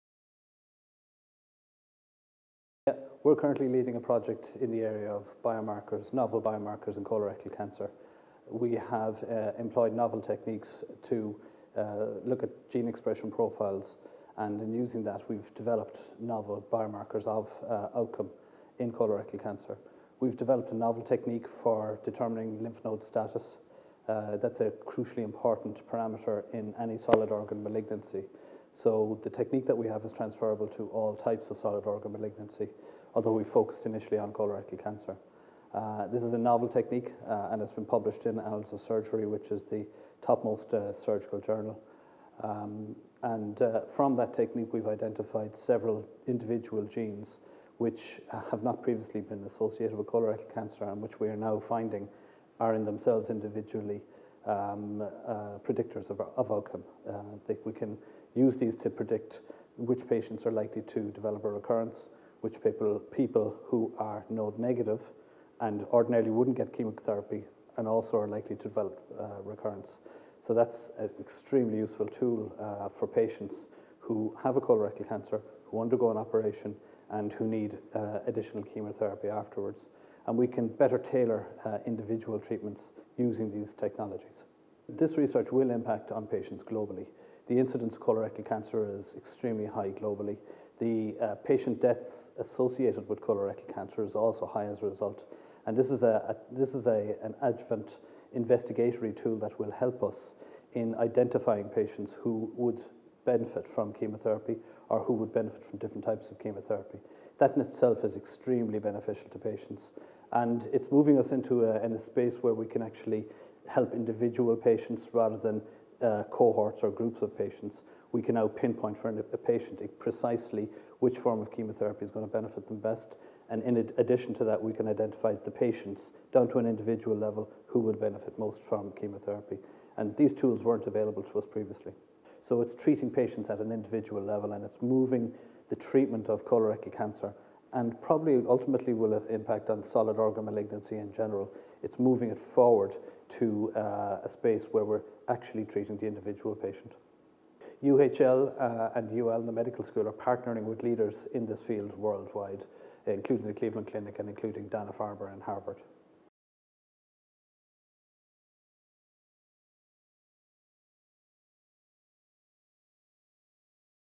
UL Research leads to better outcomes for patients with bowel cancer - Prof Calvin Coffey
Researchers at University of Limerick and University Hospital Limerick have identified several new genetic biomarkers which better predict outcomes for patients with bowel/colorectal cancer. The research team identified genes that are predictors of cancer recurrence and can also help to identify a patients’ suitability to specific types of chemotherapy.
Professor J. Calvin Coffey Graduate Entry Medical School, UL and Colorectal Surgeon, University Hospital Limerick explains: “One of the key early events in the spread of cancer is its spread to involve the lymph glands that drain the colon.   The identification of tumours that will spread to the glands is a key challenge for clinicians as these are the patients most likely to benefit from chemotherapy.  The ability to avoid harmful chemotherapeutic side-effects is a clinical need that has yet to be met by the diagnostic tools available to clinicians. In Ireland colorectal cancer is the 3rd most common cancer with  2435 new cases diagnosed each year. This diagnostic instrument that we have developed, and this research in general, will impact on patients globally as we can now pin-point precisely patients who will develop spread to glands, and thus benefit from chemotherapy.”